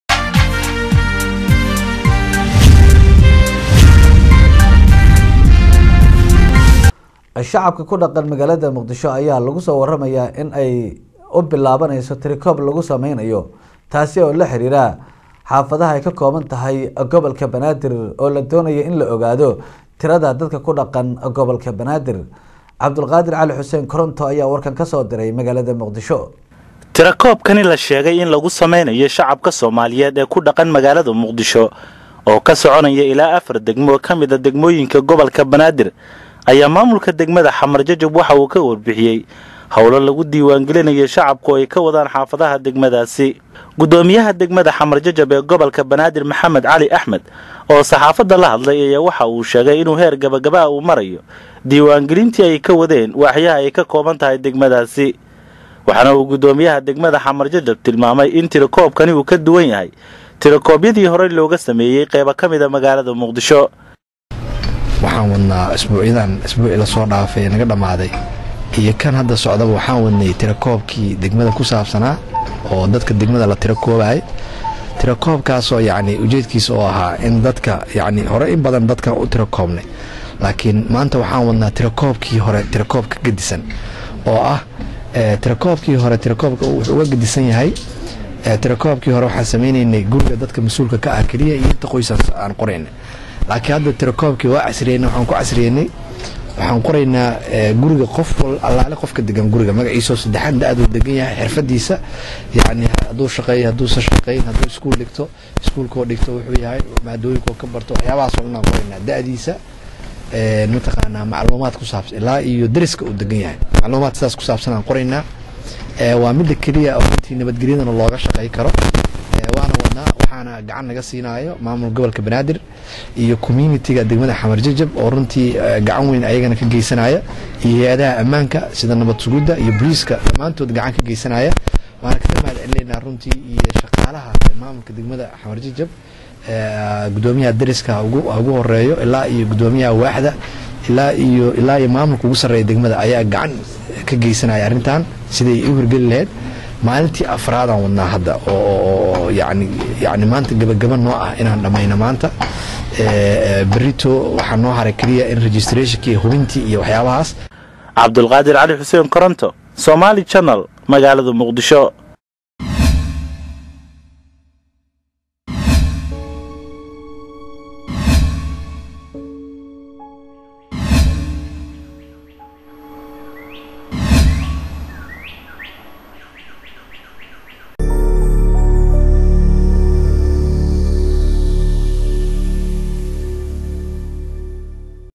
TIRA KOOB CUSUB OO LAGU SAMEYNAYO SHACABKA KU DHAQAN GOBALKA BANAADIR OO KA SOCONAYO QEYBO KA MID AH
TIRA KOOB CUSUB OO LAGU SAMEYNAYO SHACABKA KU DHAQAN GOBALKA BANAADIR OO KA SOCONAYO QEYBO KA MID AH MAGALADA MUQDISHO.